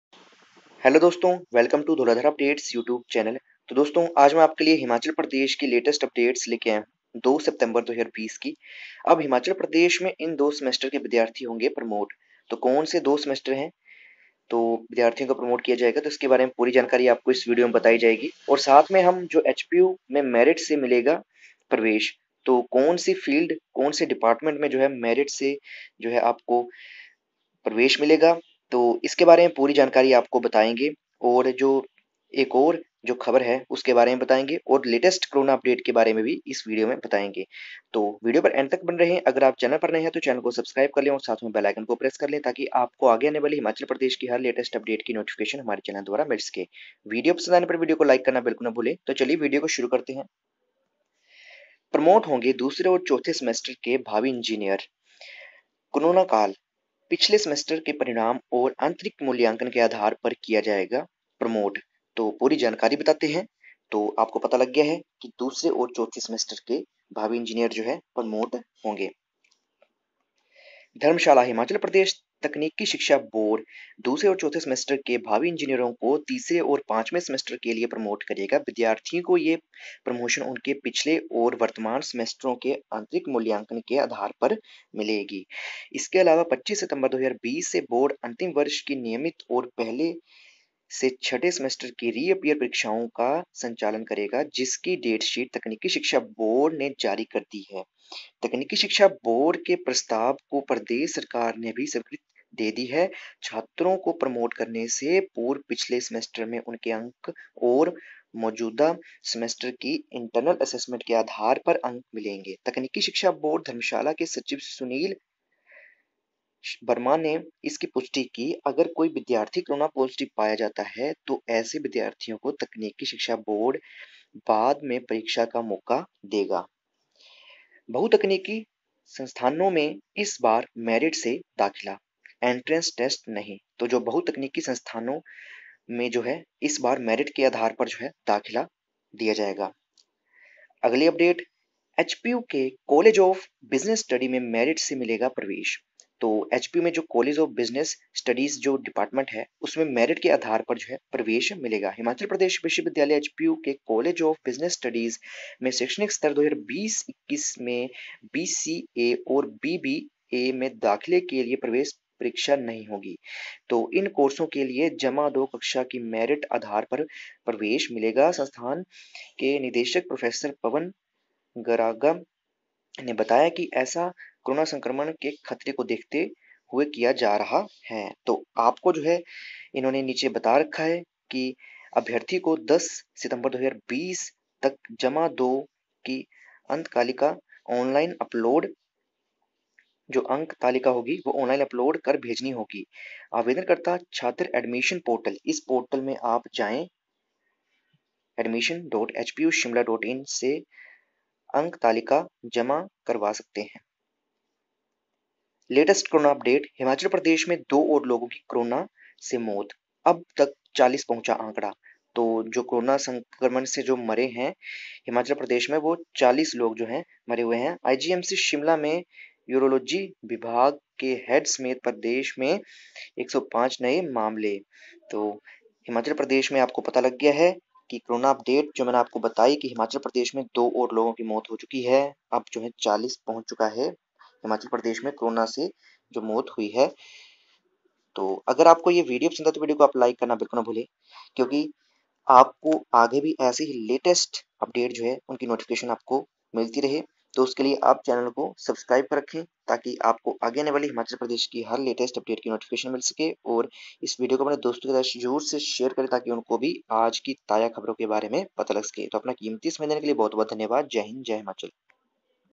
HP Latest Updates Today | अब हिमाचल में इन 2 Semester के विद्यार्थी होंगे Promote | HPU Latest News
2 September 2020 All HP Latest Updates